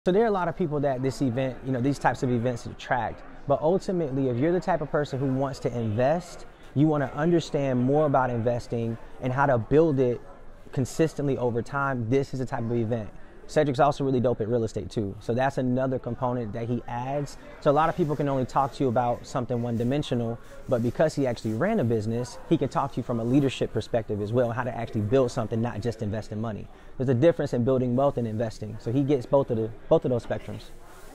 How to build wealth
Econix host Joshua bent highlights his favorite moments from  Cedric Nash's "Why Should White Guys Have All The Wealth"  Book Signing

Check out threemindsproductions.com for our full list of videography services ✅🎥🔥